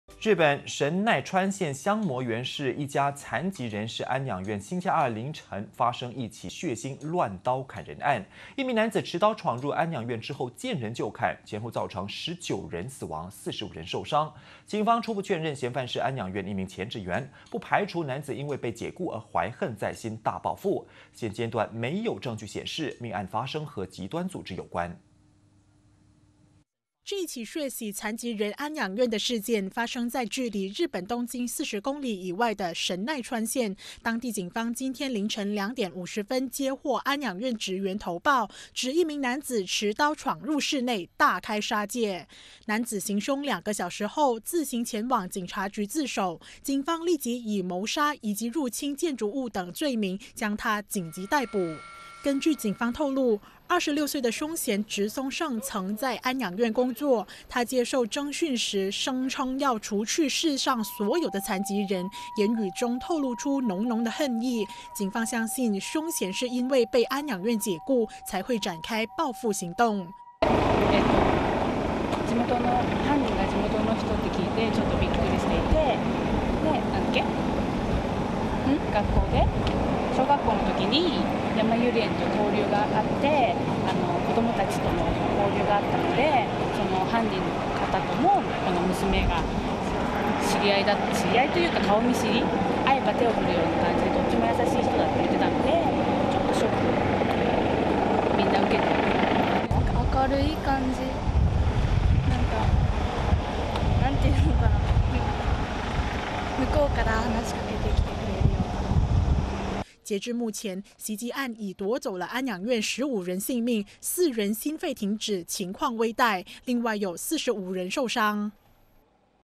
夜闯安养院大报复 日本男子连杀19人45人伤 [20160726]
日本神奈川县相模原市一家残疾人士安养院星期二凌晨发生一起血腥乱刀砍人案。一名男子持刀闯入安养院后见人就砍，前后造成19人死亡、45人受伤，警方初步确认嫌犯是安养院一名前职员，不排除男子因为遭解雇而怀恨在心，大报复。现阶段没有证据显示，命案发生与极端组织有关。